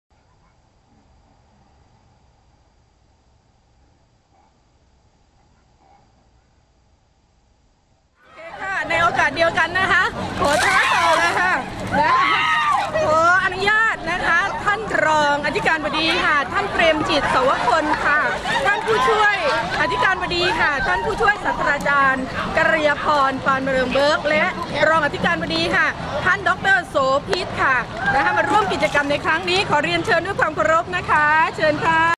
ผศ. ดร. อัจฉราพร ม.ศรีปทุม รับคำท้า ICE BUCKET CHALLENGE
ผศ. ดร. อัจฉราพร โชติพฤกษ์
ผู้อำนวยการกลุ่มงานมาตรฐานและบริการการศึกษา
ม.ศรีปทุม รับคำท้า ICE BUCKET CHALLENGE

และ ส่งต่อบุญ ให้กับ

1. อ. เปรมจิต เสาวคนธ์  รองอธิการบดี ม.ศรีปทุม

2. ผศ. กัลยาภรณ์ ปานมะเริง เบอร์ค ผช.อธิการบดี ม.ศรีปทุม

3. ดร. โสภิต ภาโนมัย   รองอธิการบดี  ม.ศรีปทุม